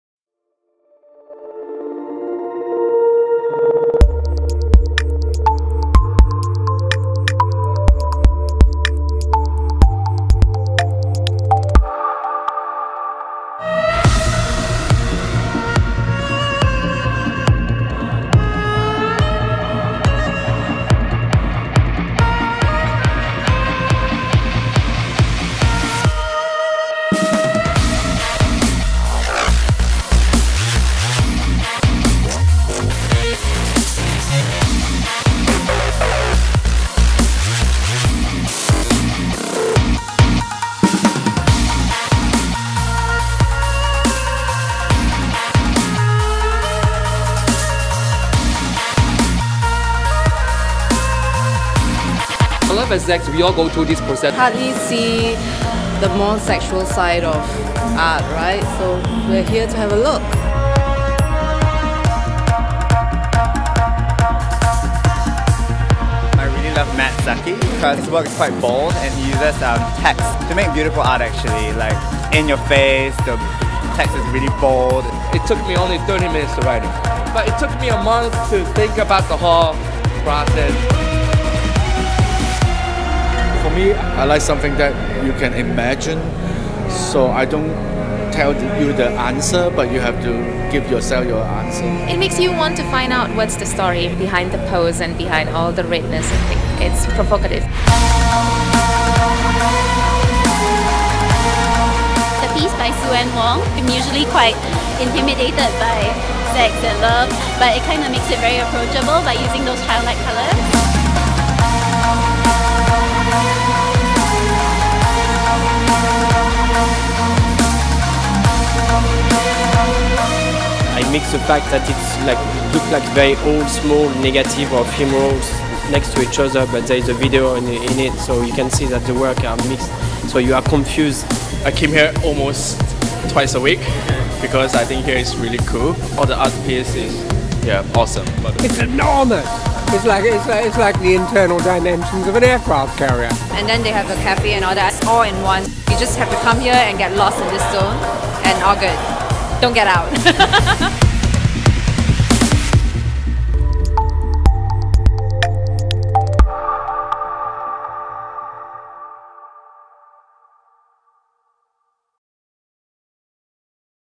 MAD Love: An Erotic Exhibition
The subject of love is an age-old adage and one of the most confounding and universal expressions of the human condition. The exhibition invites artists to interpret and recontextualize the subject and extremities of love, sex and desire in these contemporary times, resulting in an expressive breath of works which begs the questions: If love knows no boundaries, just how far or maddening can it get?